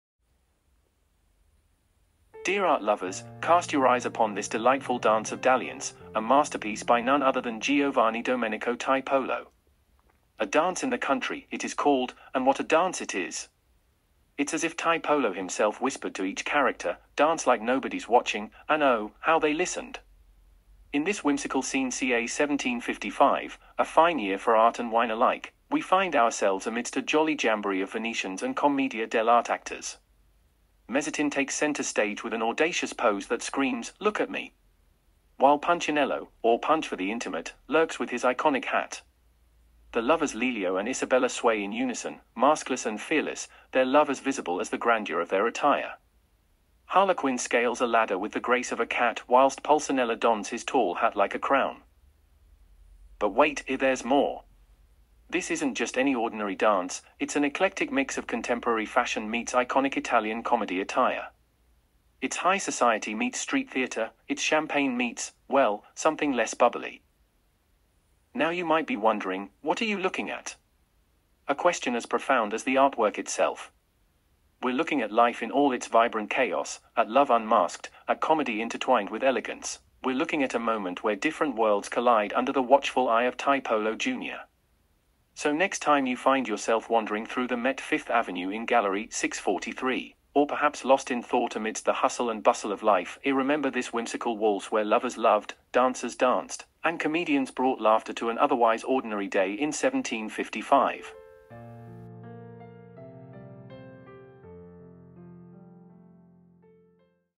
Dance like nobody’s watching through The Met Fifth Avenue in Gallery 643
Unlike his father, the younger Tiepolo did not pursue complex allegorical schemes, but expressed his artistic personality most strongly through idealized depictions of contemporary life. This country dance combines figures dressed fashionably and in the archetypal costumes of the Italian comedy, including the jaunty figure of Mezzetin at center and the masked, hatted character of Punchinello, or Punch, who was a favorite of the artist. Though the mood is very different from the elder Tiepolo’s paintings, similarly unexpected and playful tricks of composition and dramatic foreshortening appear here, such as the chair turned away at right and Mezzetin’s incredible pose: raised on one leg with his elbow pointed toward the viewer. A traveling troupe of commedia dell'arte actors entertains a party of Venetians enjoying a summer holiday on the mainland. Such performances often ended with a minuet, depicted here. The dancing couple may be the lovers Lelio and Isabella (sometimes called by other names), the only actors in the troupe who did not wear masks. Recognizable in the crowd of onlookers are other Commedia dell’arte characters: Columbine, the masked woman behind the dancers; Pulcinella, the man in white at center with a crooked nose and tall hat (a second Pulcinella is visible behind him); the Doctor, the sinister figure in a black robe and floppy hat; Coviello, the bass player at the right with feathers sprouting from his hat; Harlequin, the acrobat climbing the ladder leaning against one of the trees; and, possibly, Pasquariello, the masked man wearing a ruff and a close-fitting cap talking to the woman seated on the right.